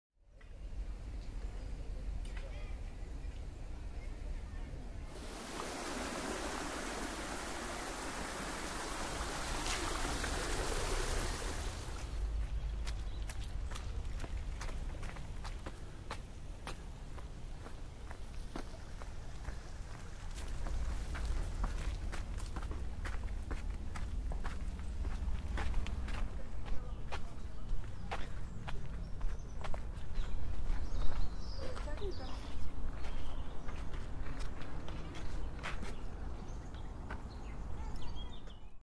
Barcelona Horta's Labyrinth Park Preview
This tour will take you through the Horta's Labyrinth Park (Parc del Laberint d’Horta). It’s located in the Horta and Guinardó district of Barcelona, Spain.
Get lost in Barcelona's oldest garden. Sound like fun? This might be the tour for you!